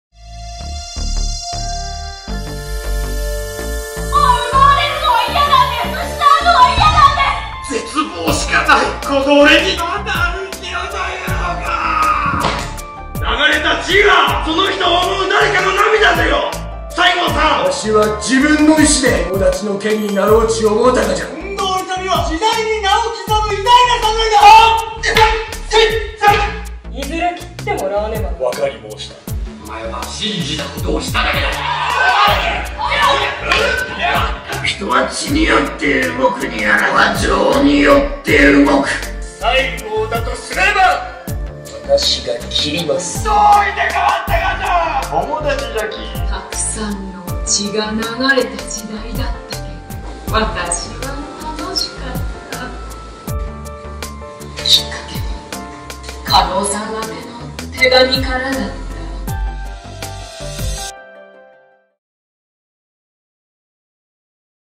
舞台「君死にたまふことなかれ」PR 編
劇団「ア・レディーバードシアターカンパニー」が2017年８月公演「君死にたまふことなかれ」は同劇団史上最多客演数！その役者陣の多彩な顔ぶれも見所のひとつ！